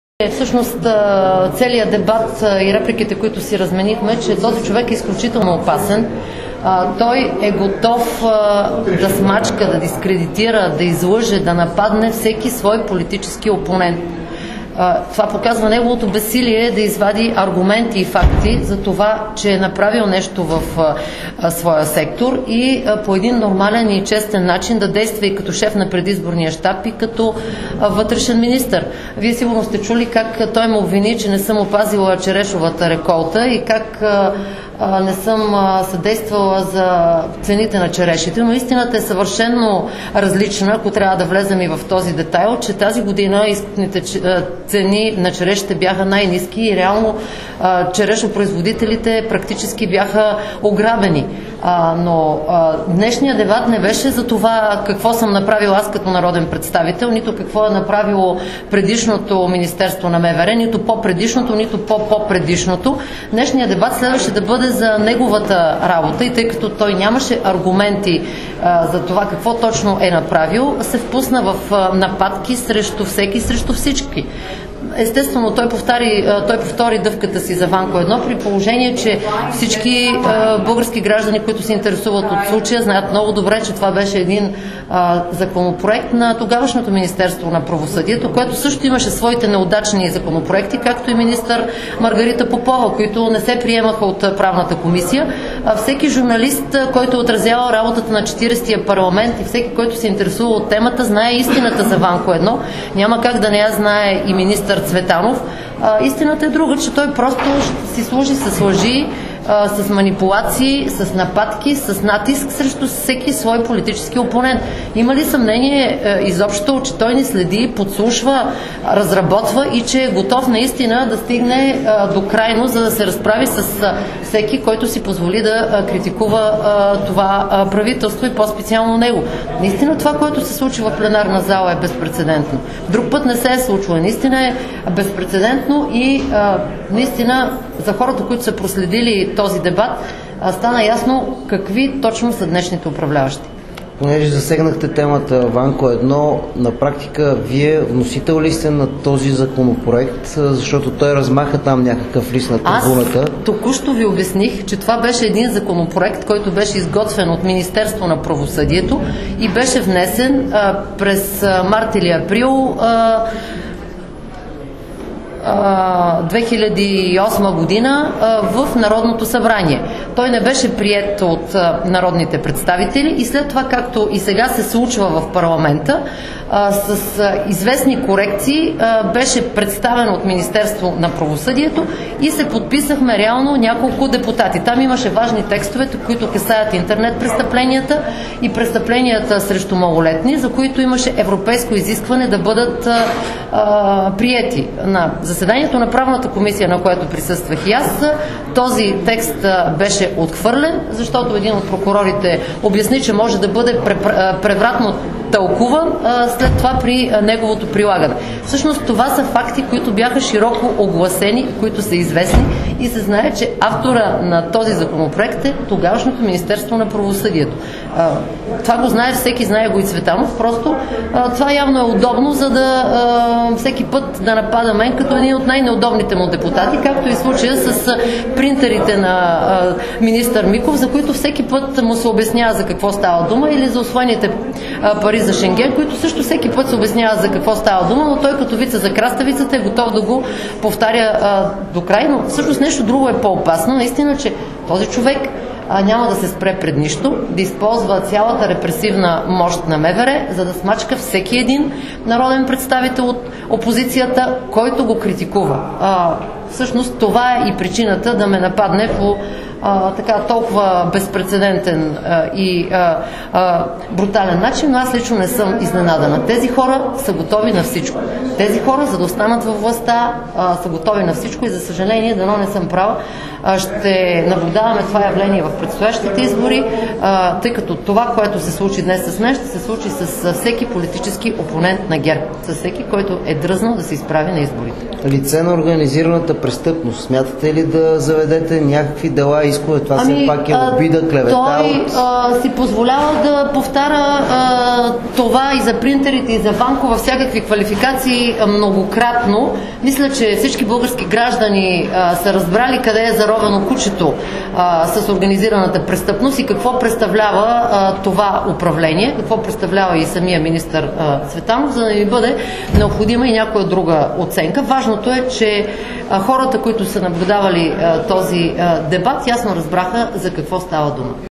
Цветанов - обвинения - Манолова
Дебатът днес показа тоталният провал в сектора за сигурност. Това показа и, че този човек (Цветан Цветанов - бел. ред.) е изключително опасен. Той е готов да смачка, да дискредитира, да излъже, да нападне всеки свой политически опонент. Това заяви народният представител от ПГ на Коалиция за България Мая Манолова пред Пресклуб ИА (PIA) след края на дебатите в НС по внесения вот на недоверие.